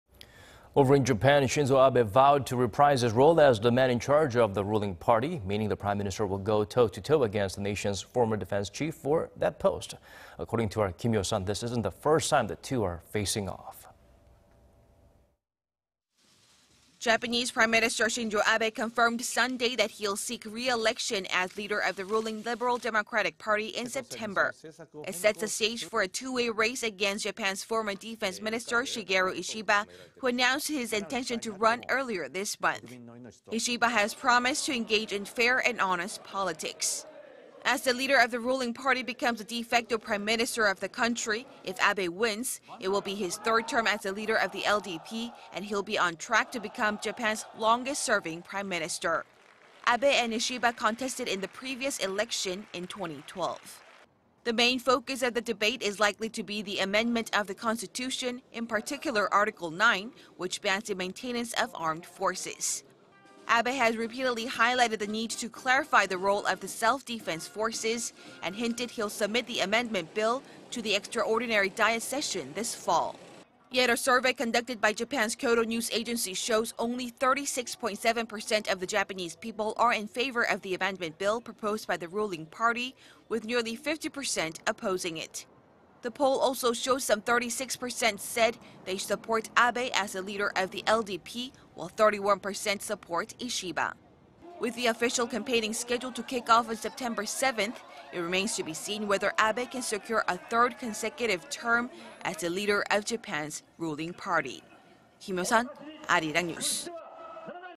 Japanese PM seeks re-election as ruling party leader
아베, 총재선거 출마 선언... 이시바와 두번째 맞대결
  Over in Japan.
Shinzo Abe vowed to reprise his role as the man in charge of the ruling party.
Meaning the prime minister will go toe to toe against the nation's former defense chief for that post.
According to our Kim Hyo-sun this isn't the first time the two are facing off.
  Japanese Prime Minister Shinzo Abe confirmed Sunday that he'll seek re-election as leader of the ruling Liberal Democratic Party in September.
It sets the stage for a two-way race against Japan's former Defense Minister Shigeru Ishiba,... who announced his intention to run earlier this month.
Ishiba has promised to engage in fair and honest politics.
As the leader of the ruling party becomes the de facto prime minister of the country,... if Abe wins,... it will be his third term as the leader of the LDP, and he'll be on track to become Japan's longest-serving prime minister.
Abe and Ishiba contested in the previous election in 2012.
  The main focus of the debate is likely to be the amendment to the Constitution,... in particular,... Article 9,... which bans the maintenance of armed forces.
Abe has repeatedly highlighted the need to clarify the role of the Self-Defense Forces,... and hinted he'll submit the amendment bill to the extraordinary Diet session this fall.
Yet,... a survey conducted by Japan's Kyodo News Agency shows only 36-point-seven percent of the Japanese people are in favor of the amendment bill proposed by the ruling party,... with nearly 50 percent opposing it.
The poll also showed some 36 percent said they support Abe as the leader of the LDP while 31 percent support Ishiba.
  With the official campaigning schedule to kick off on September 7th,... it remains to be seen whether Abe can secure a third-consecutive term as the leader of Japan's ruling party.